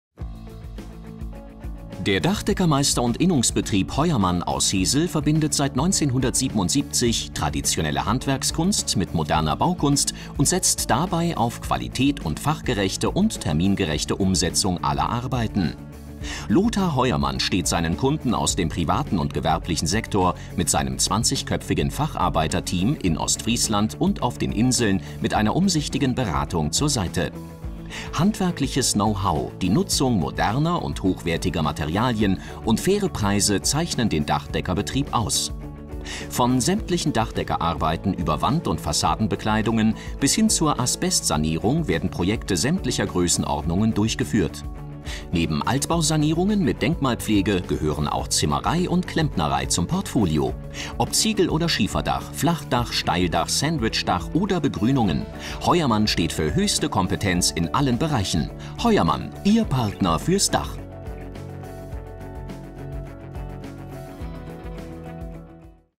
Dachdecker H.-D. Heuermann GmbH | Unternehmensfilm
Der Dachdeckermeister- und Innungsbetrieb Heuermann aus Hesel wurde 1977 gegründet. Das zertifizierte Familienunternehmen verbindet traditionelle Handwerkskunst mit moderner Baukunst und setzt dabei auf Qualität und die fachgerechte, solide und termingerechte Umsetzung aller Arbeiten.

Lothar Heuermann steht seinen Kunden aus dem privaten und gewerblichen Sektor mit seinem 20-köpfigen Facharbeiter-Team in Ostfriesland und auf den Inseln mit einer umsichtigen Beratung und einem umfangreichen Leistungsspektrum zur Seite. Handwerkliches Know-how, die Nutzung moderner und hochwertiger Materialien und faire Preise zeichnen den Dachdeckerbetrieb aus. Von sämtlichen Dachdeckerarbeiten über Wand- und Fassadenbekleidungen bis hin zur Asbestsanierung werden Projekte sämtlicher Größenordnungen durchgeführt. Neben Altbausanierungen mit Denkmalpflege gehören auch Zimmerei und Klempnerei zum Portfolio.

Ob Ziegel- oder Schieferdach, Flachdach, Steildach, Sandwichdach oder Begrünungen Heuermann steht für höchste Kompetenz in allen Bereichen.

Heuermann – Ihr Partner fürs Dach.